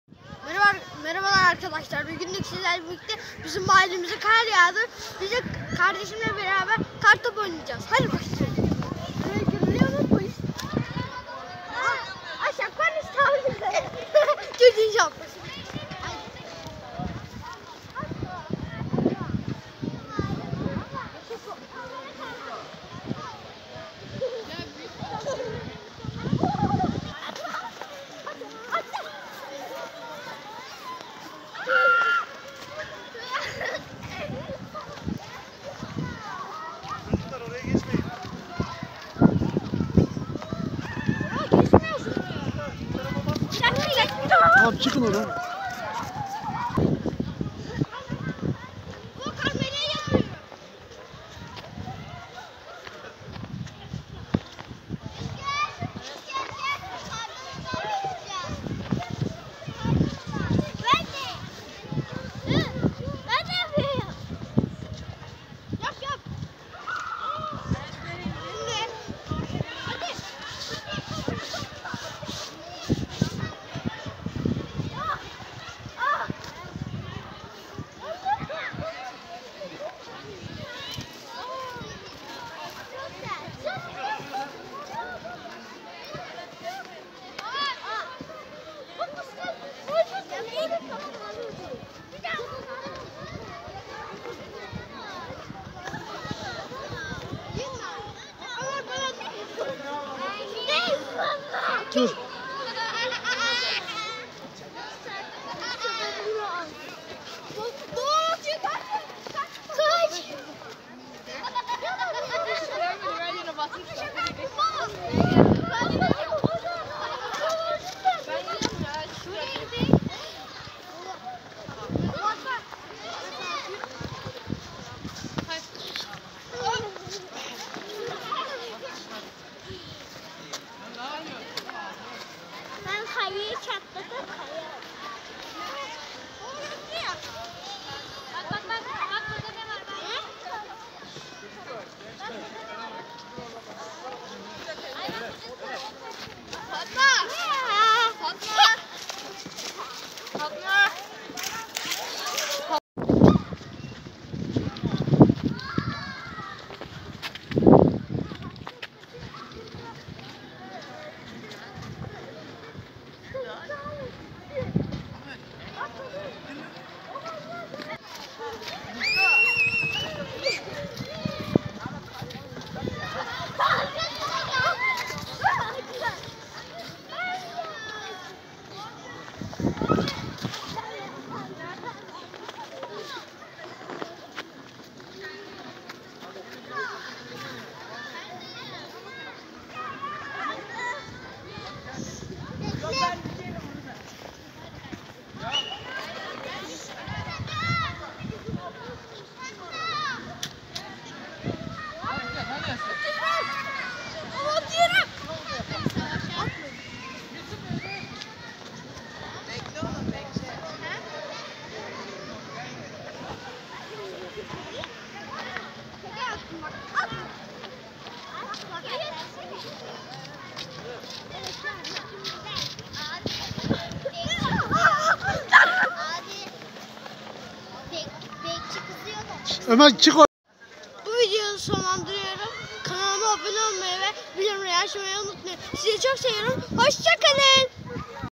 KAR YAĞDI, ÇOK MUTLUYUZ, KARTOPU OYNADIK, İstanbul' dan kar manzaraları,
HAMZA - ÖMER ALEMDAR KARDEŞLER. hamza ile ömer youtube